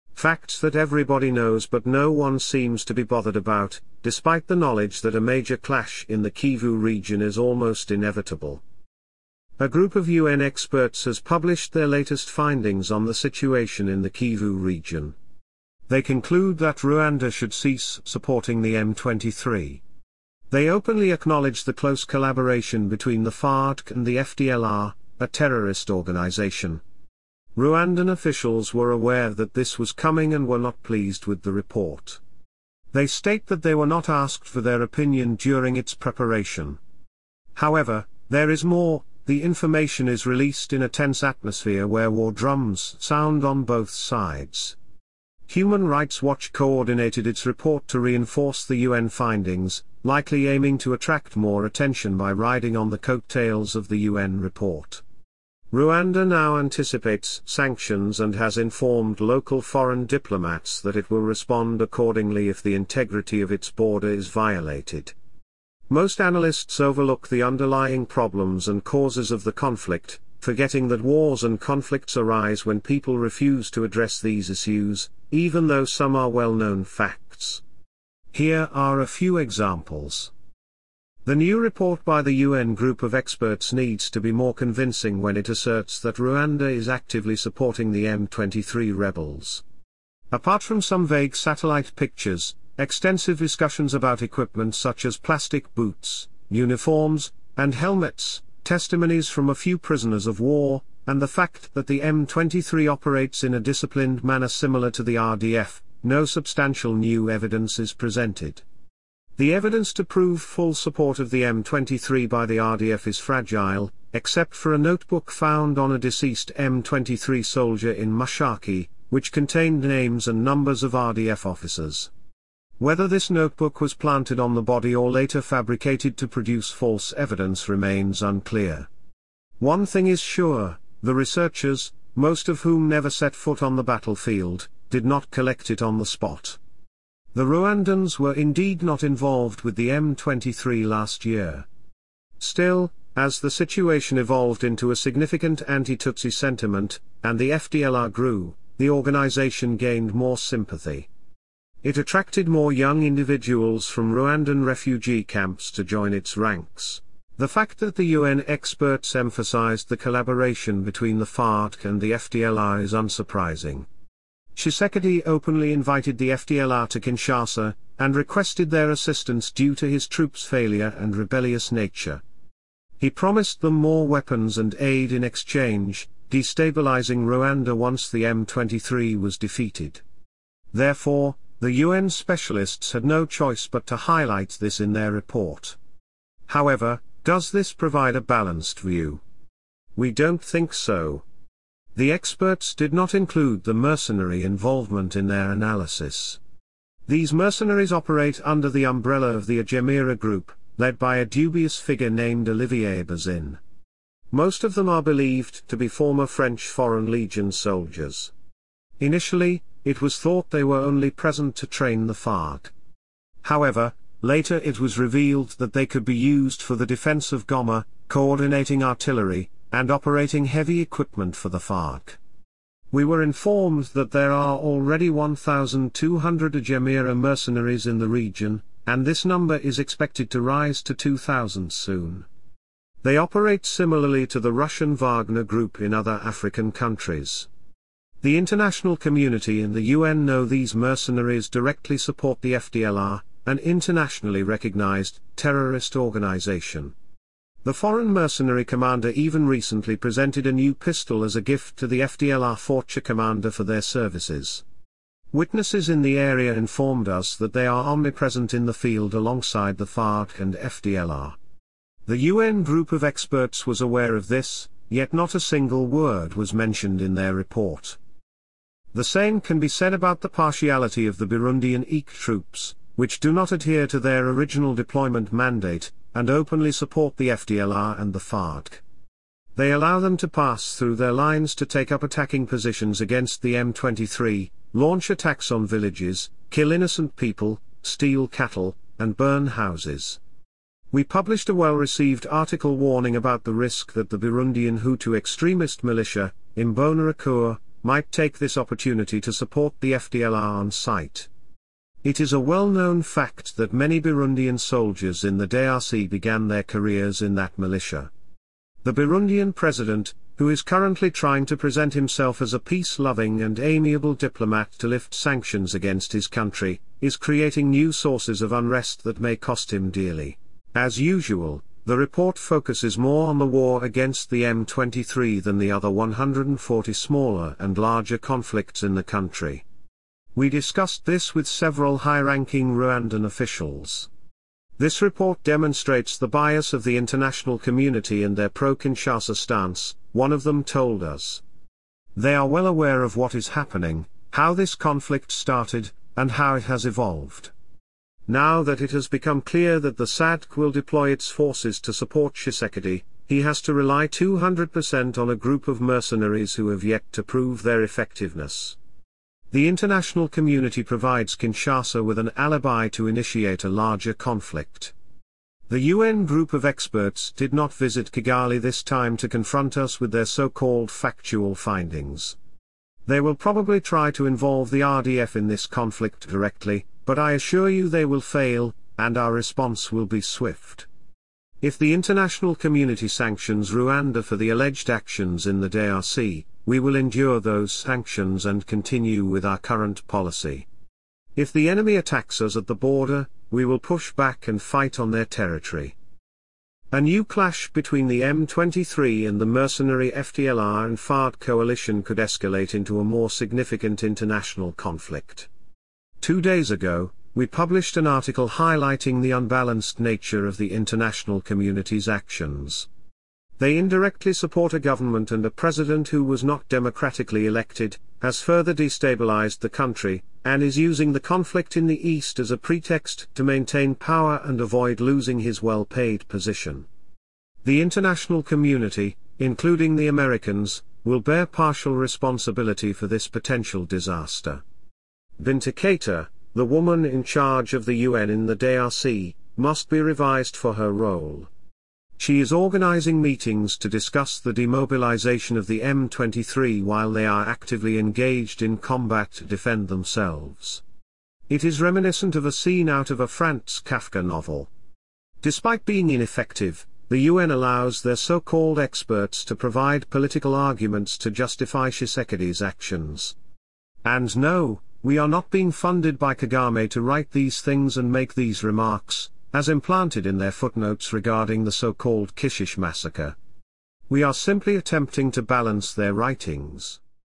Kagame's Role Exposed: RDF & M23 Implicated in Kivu Conflict
In this eye-opening video, we delve into a highly significant report released by a United Nations group of experts that sheds light on the intricate dynamics of the Kivu region and implicates Rwanda's President, Paul Kagame, and the Rwandan Defense Forces (RDF) in the looming conflict. Titled "Kagame's Role Revealed: RDF and M23 Implicated in the Looming Conflict in the Kivu Region," this report unravels a complex web of connections, highlighting the involvement of key actors and the potential consequences for the region. Join us as we analyze the findings and discuss the implications of this critical report.